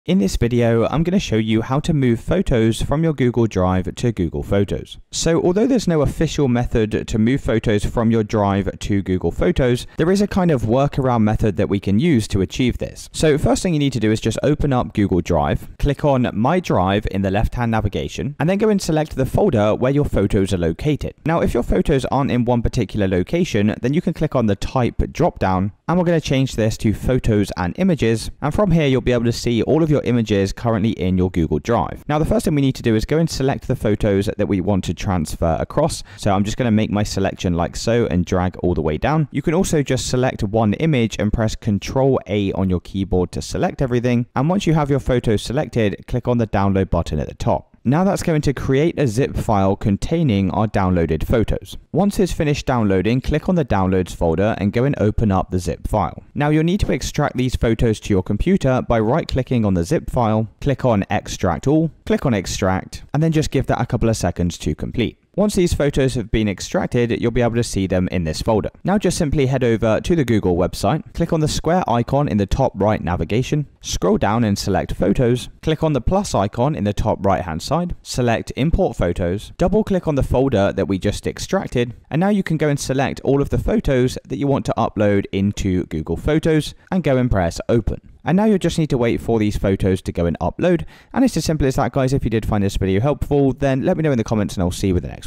How To Transfer Photos From Google Drive to Google Photos
In this video, learn how to move photos from Google Drive to Google Photos. Whether you're organizing your cloud storage, backing up images, or transferring important pictures, this guide will show you the best method to download and upload photos between the two platforms.